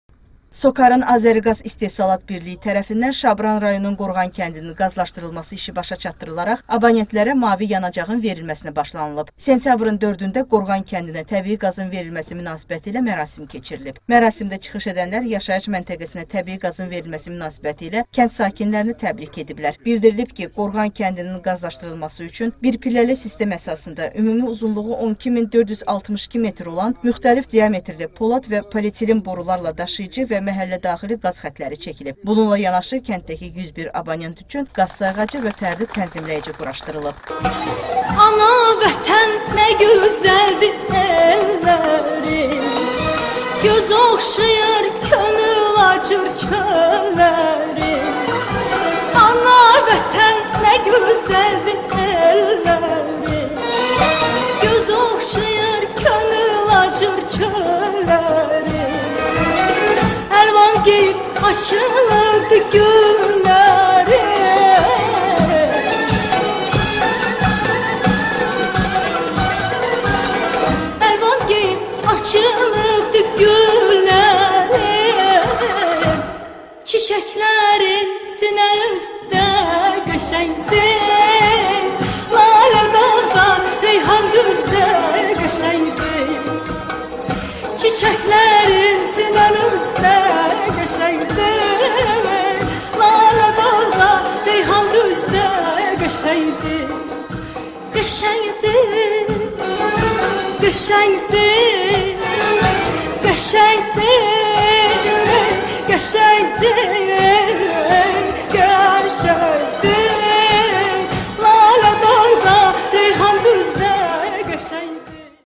Şabran Rayon Qorğan Kəndinə Təbii Qaz Verilib
SOCAR-ın “Azəriqaz” İstehsalat Birliyi (İB) tərəfindən Şabran rayonunun Qorğan kəndinin qazlaşdırılması işi başa çatdırılaraq abonentlərə “mavi yanacağ”ın verilməsinə başlanılıb. Sentyabrın 4-də Qorğan kəndinə təbii qazın verilməsi münasibətilə keçirilən mərasimdə rayon icra hakimiyyətinin və “Azəriqaz” İB-nin nümayəndələri, ziyalılar, eləcə də kənd sakinləri iştirak ediblər. Mərasimdə çıxış edənlər yaşayış məntəqəsinə təbii qazın verilməsi münasibətilə kənd sakinlərini təbrik ediblər. Bildirilib ki, kənddəki 101 abonent üçün qaz sayğacı və fərdi tənzimləyici quraşdırılıb.
Çıxışlarda rayonun yaşayış məntəqələrinin, xüsusən də dağ kəndlərinin “mavi yanacaq”la təminatı vətəndaşlarla yanaşı, təbiətə, ətraf mühitə qayğı kimi qiymətləndirib. Qeyd olunub ki, rayonun Sumağava Qazma kəndinin də qazlaşdırılması işləri yekunlaşdırılıb. Tezliklə bu yaşayış məntəqəsinin sakinləri də “mavi yanacaq”la təmin ediləcək.
Sonra məşəl alovlandırılıb və kəndə təbii qaz verilib.